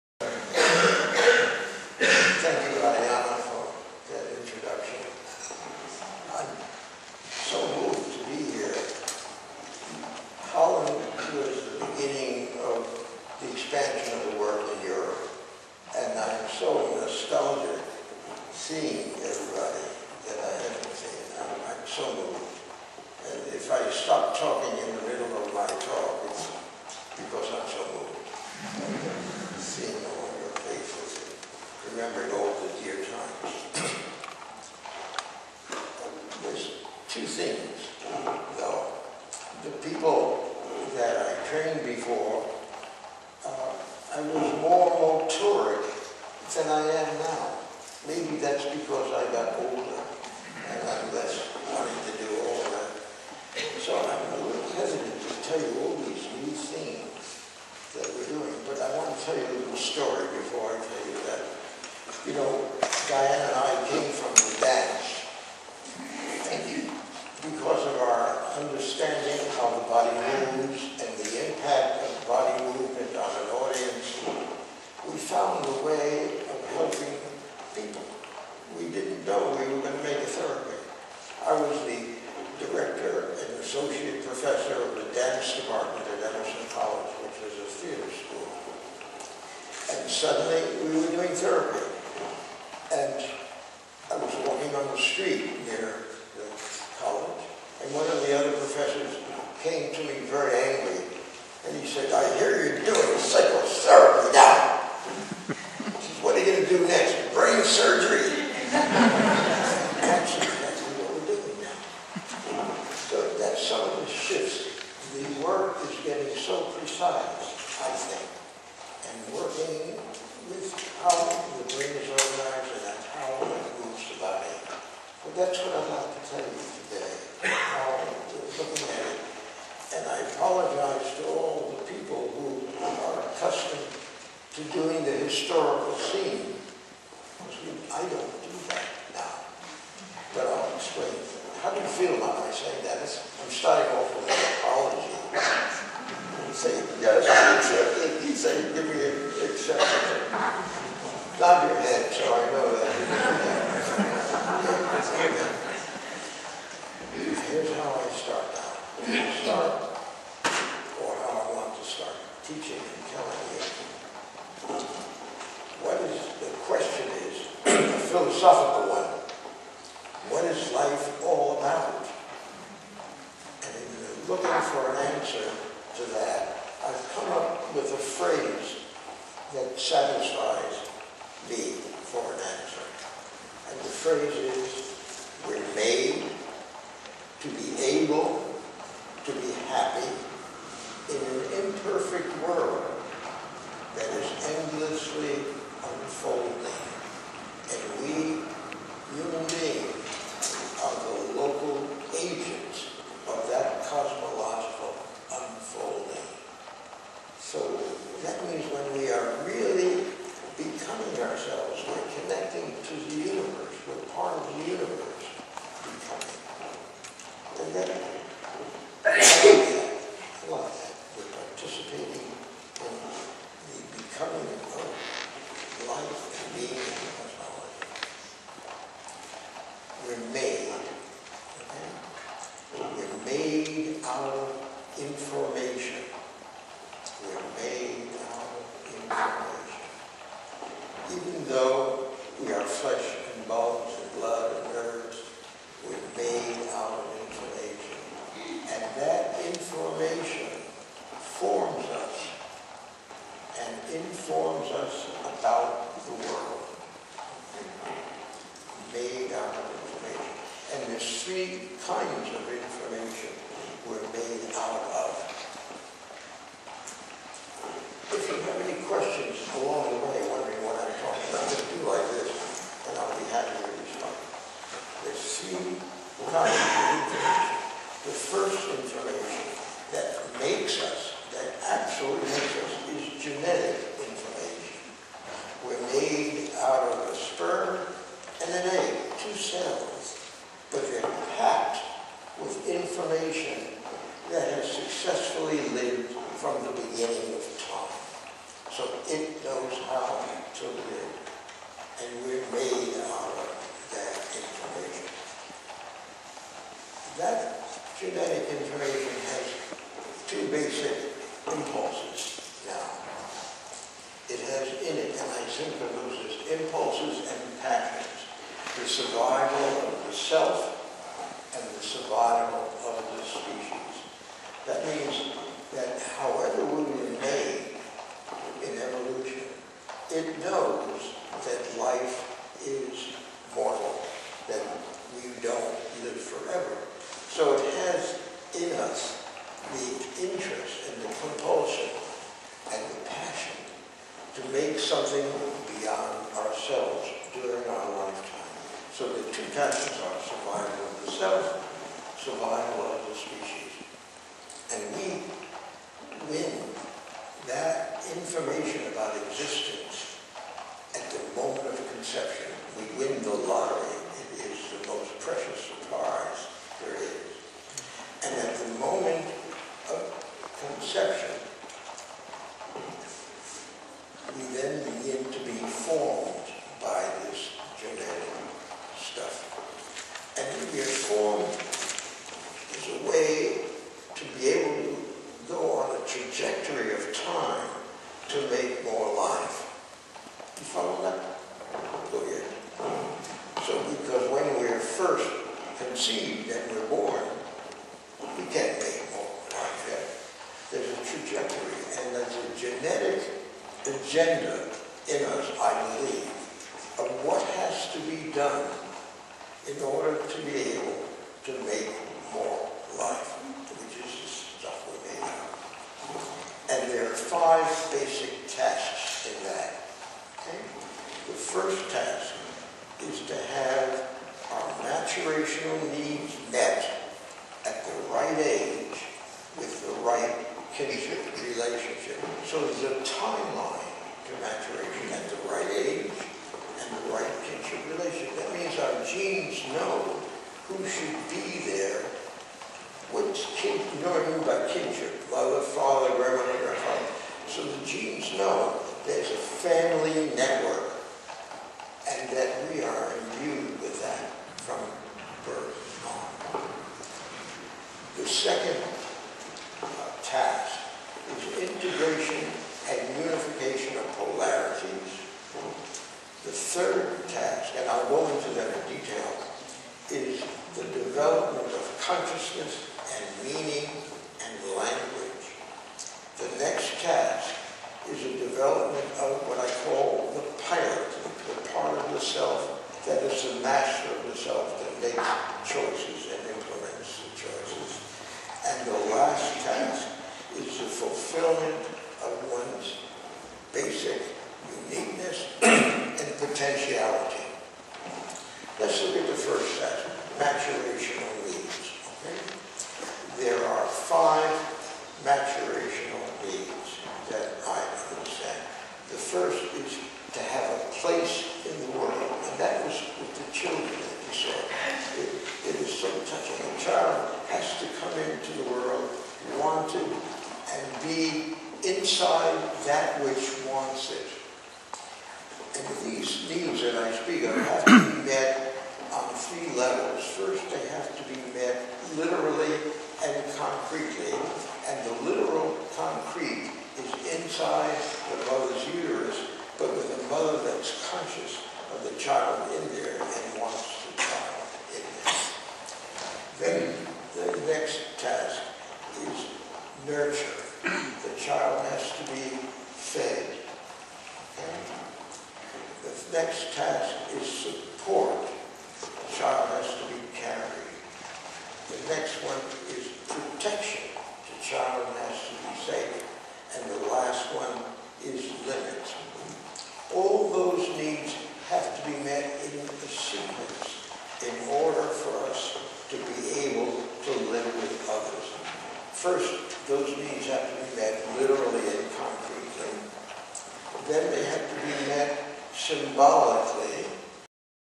Albert Pesso Utrecht Lecture Nov 2012 Part 1
Part One of Albert Pesso's introductory lecture at the Netherlands PBSP Annual Congress in Utrecht in November 2012.

Albert Pesso outlines the history, core principles and current thinking behind the PBSP (Pesso Boyden System Psychomotor) Therapy.